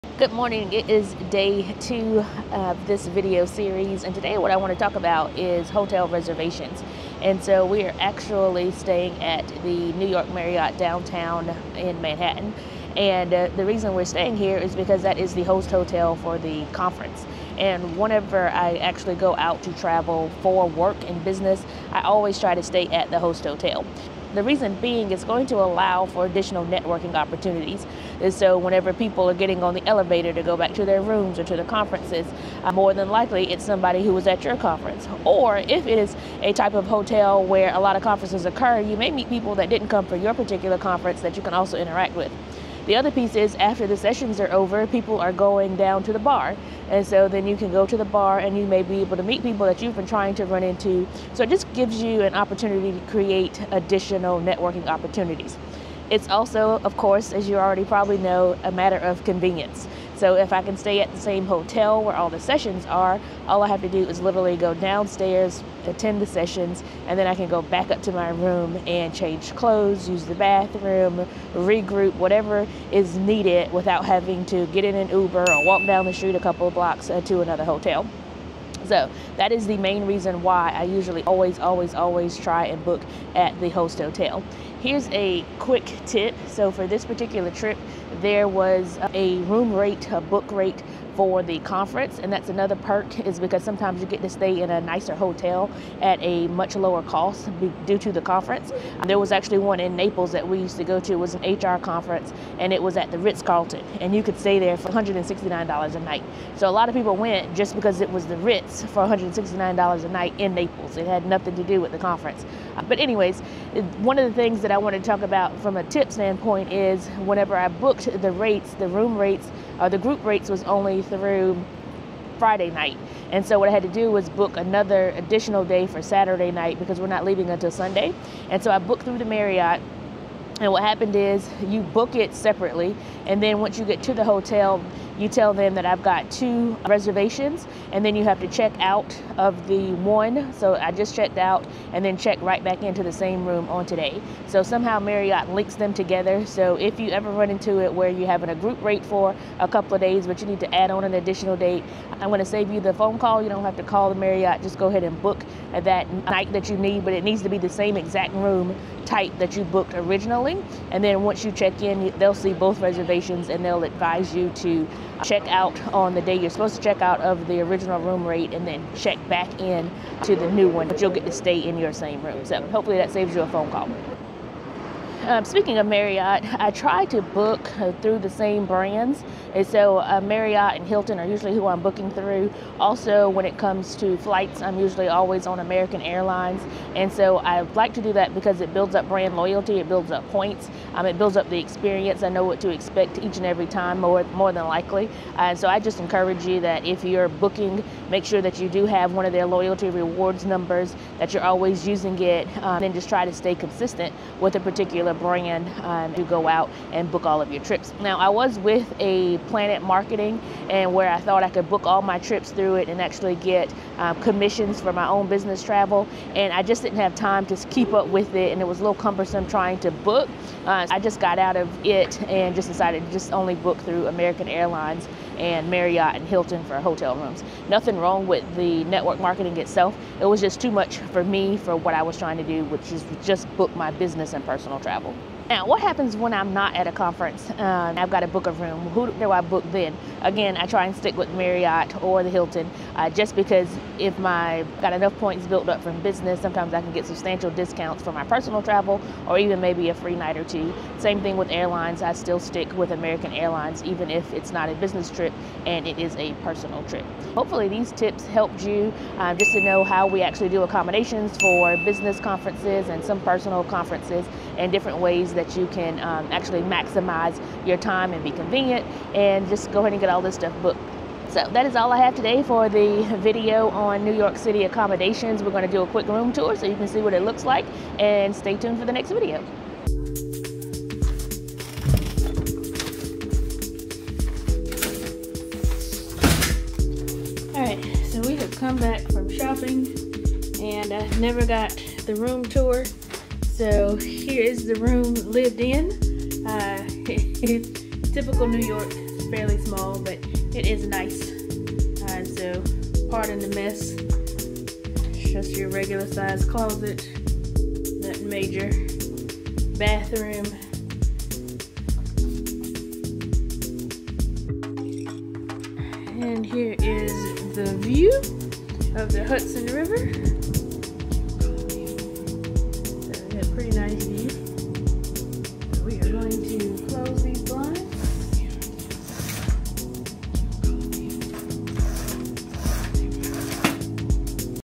Navigating Business Travel: Where to Stay & Why
Join me as I share invaluable business travel tips from my recent trip to New York City! This video covers everything from why you should always try to book at the conference host hotel to how to smartly manage your hotel bookings to extend your stay without breaking the bank. I'll give you a peek into my stay at the New York Marriott Downtown, show you how to use loyalty programs to your advantage, and even take you on a quick tour of my hotel room with a view of the Hudson River. Don't miss these tried and true strategies to enhance your travel experience!